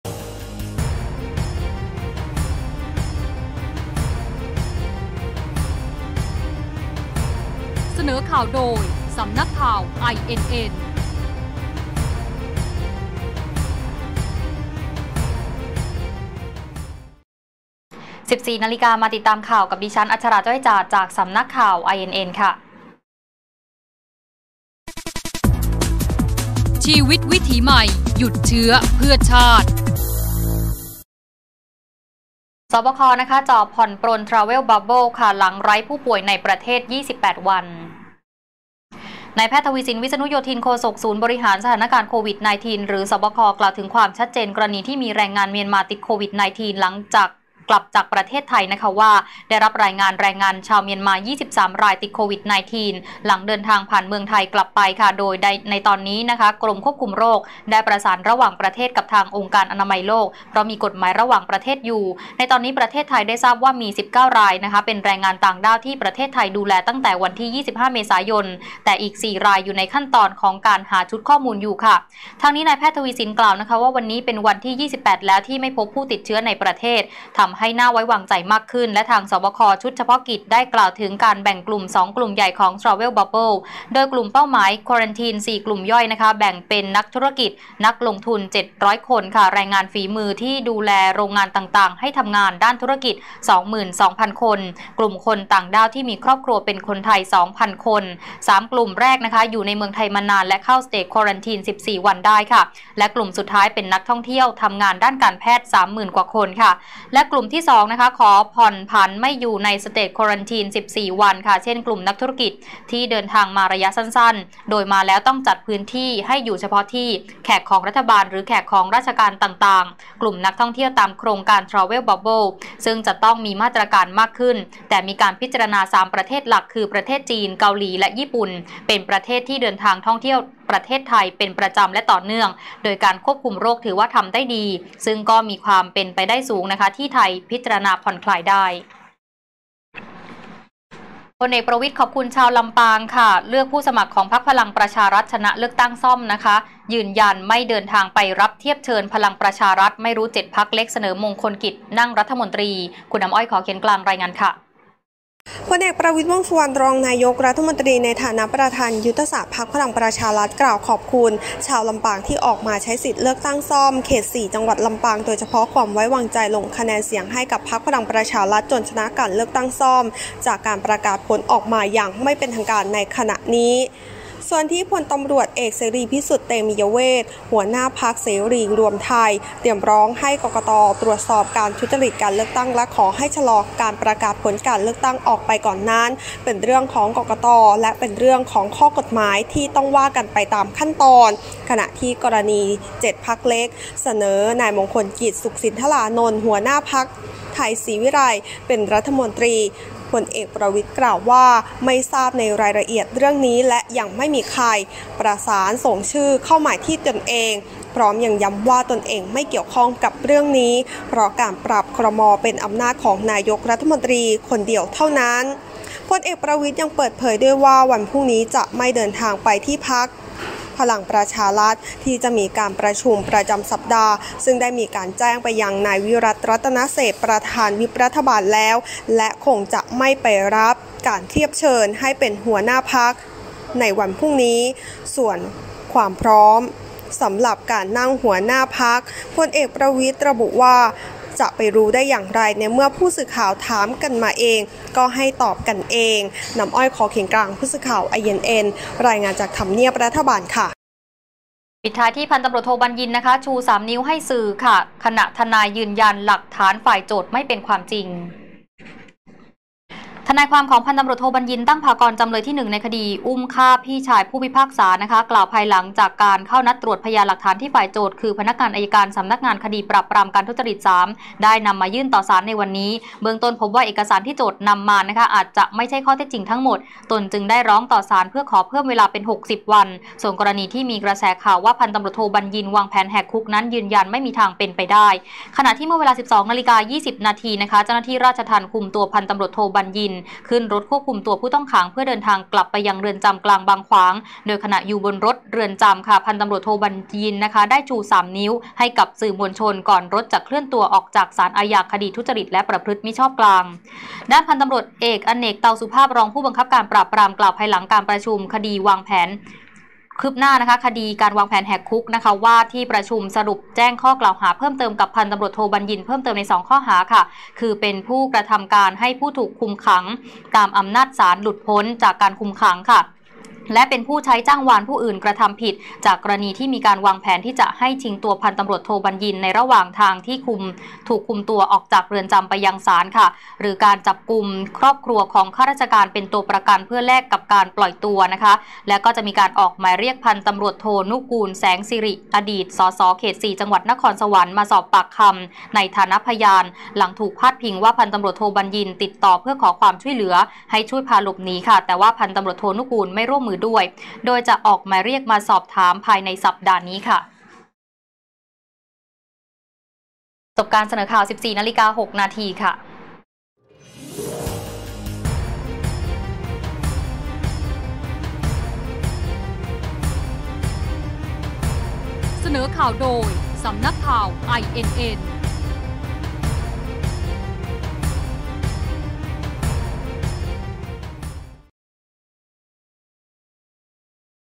"ประวิตร"ขอบคุณชาวลำปางเลือกผู้สมัครพปชร. : ข่าวต้นชั่วโมง 14.00 น. (22/06/2563)
- ศบค. จ่อผ่อนปรน Travel Bubble หลังไร้ป่วยในประเทศ 28วัน

- "ประวิตร" ขอบคุณชาวลำปาง เลือกผู้สมัครพปชร. ชนะ เลือกตั้งซ่อม ส.ส. ยืนยัน ไม่เดินทางไปรับเทียบเชิญพปชร. ไม่รู้ 7 พรรคเล็กเสนอ "มงคลกิตติ์" นั่งรัฐมนตรี

- “บรรยิน” ชู 3 นิ้ว ให้สื่อ ก่อนกลับเรือนจำ หลังศาลอนุญาต เลื่อนนัดตรวจหลักฐาน ไปอีก 60 วัน - ทนายยัน หลักฐานฝ่ายโจทย์ไม่เป็นความจริง